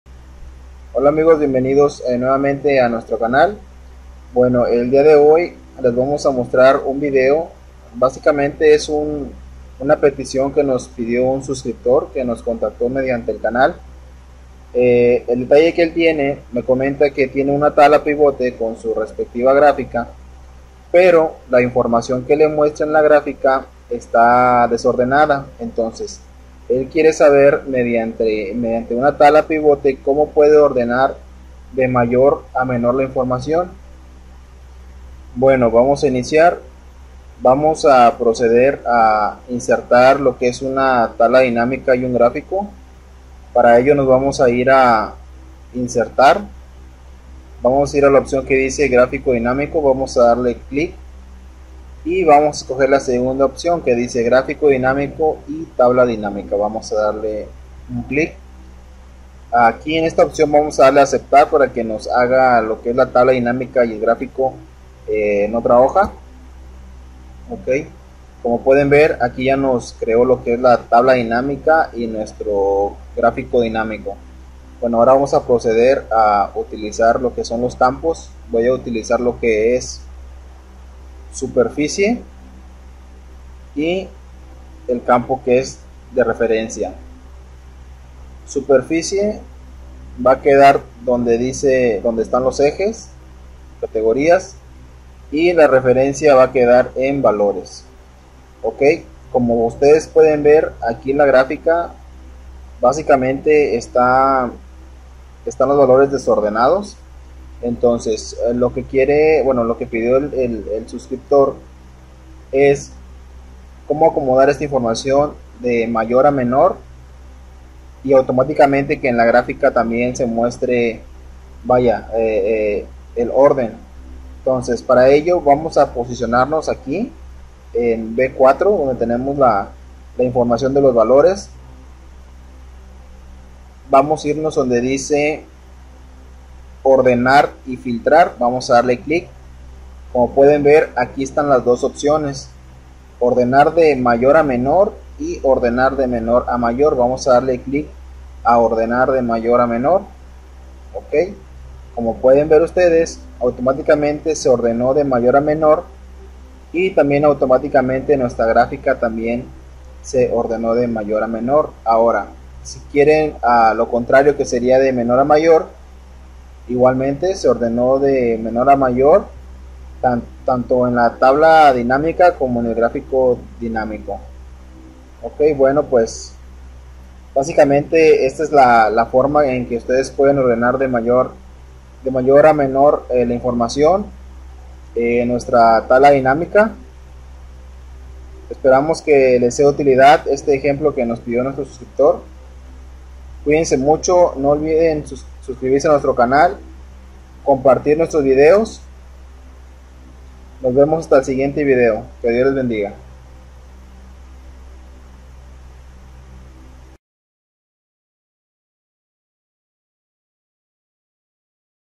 Como ordenar de mayor a menor en una tabla pivote (Tabla Dinamica)
Ordena de Mayor a Menor o Menor a Mayor tu información según sea tu necesidad!